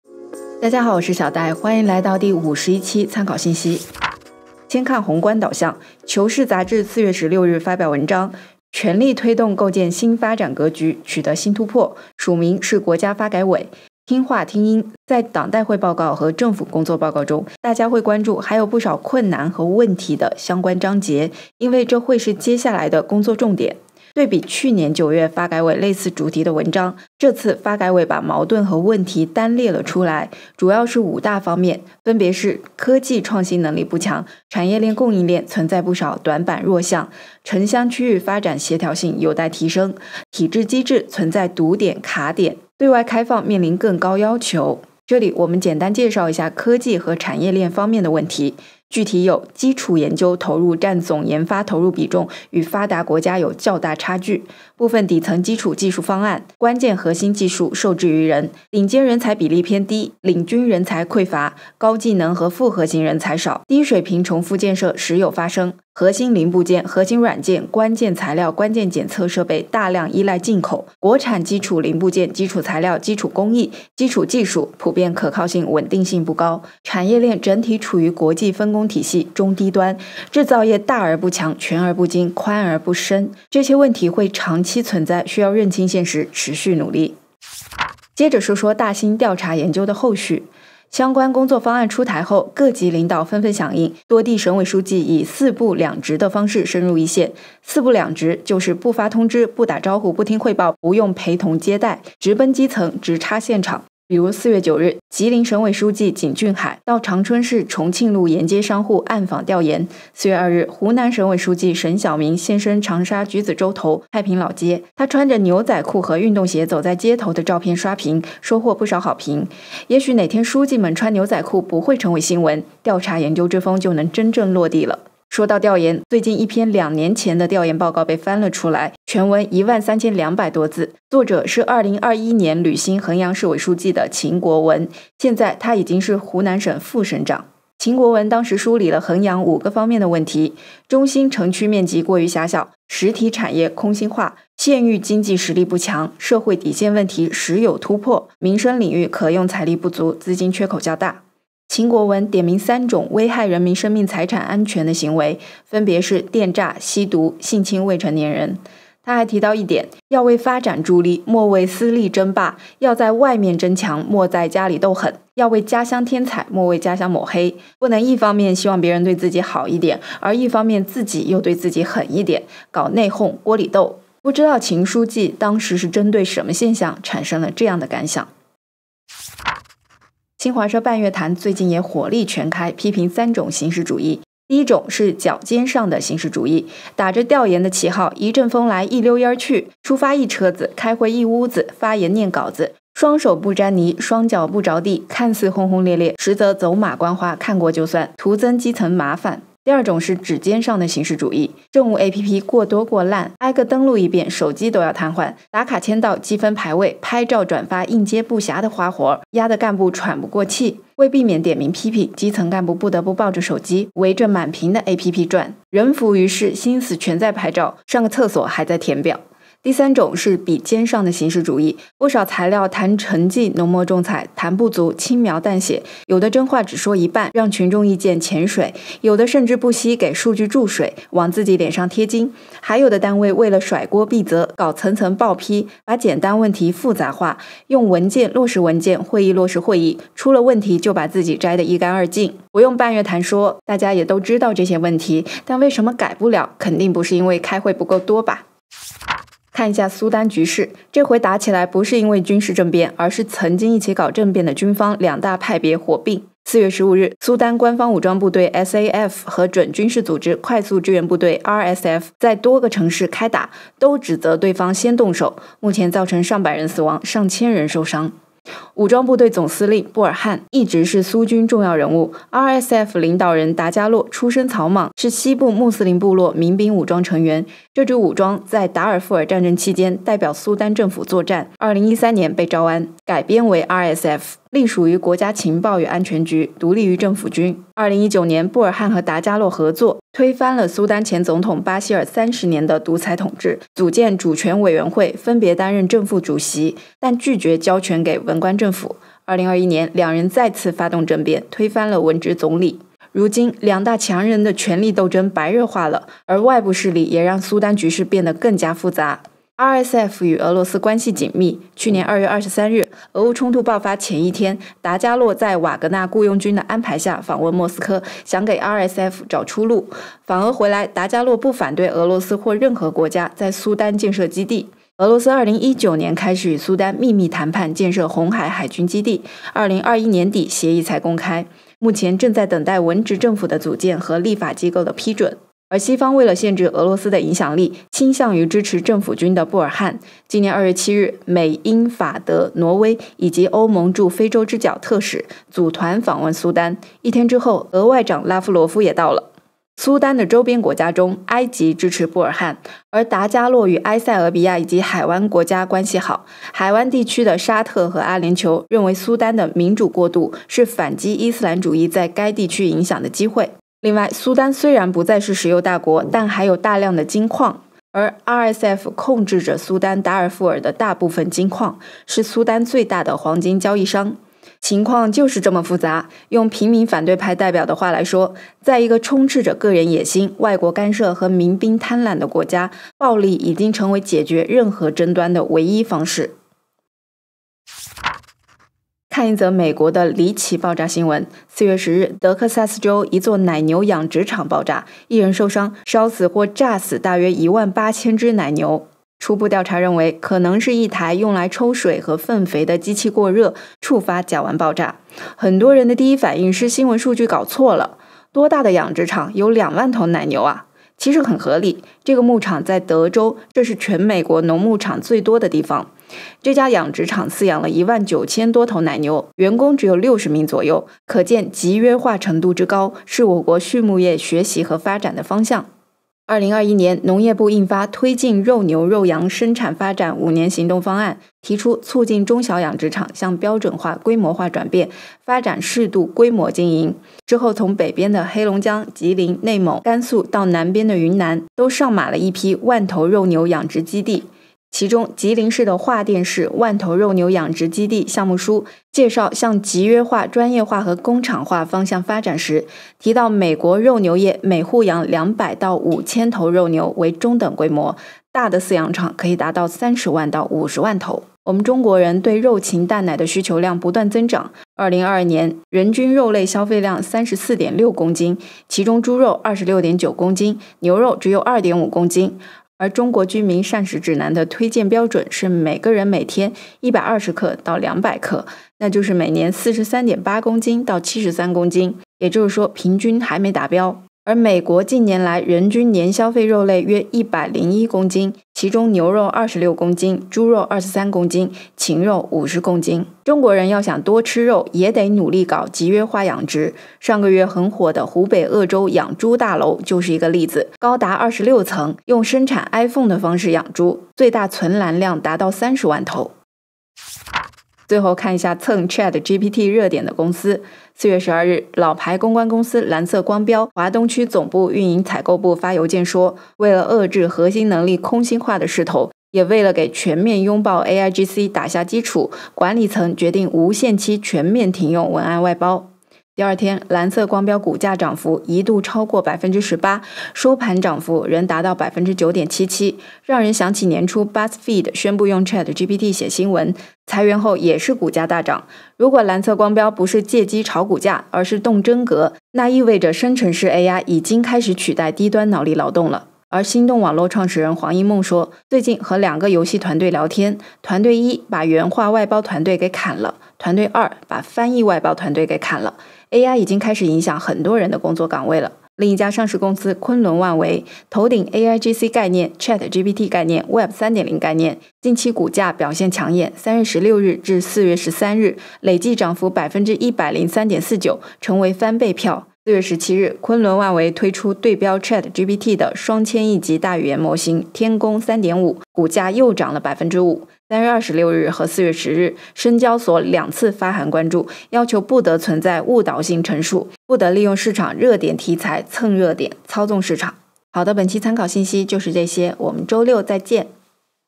【参考信息第51期】火力全开半月谈 ；俄美阿博弈苏丹
《半月谈》最近批评了三种形式主义，分别是“脚尖上的形式主义”“指尖上的形式主义”“笔尖上的形式主义”，痛快是痛快，大家也都知道这些问题，困难在于为什么改不了。苏丹又打起来了，但不是军事政变，是曾经一起合伙搞政变的军方两大派别火并，一边背后有美国、欧洲和埃及，一边背后是俄罗斯和海湾国家，很复杂。美国养了近2万头奶牛的养殖场爆炸，这个奶牛场只有60人，这种集约化工厂化畜牧业，是值得学习的方向。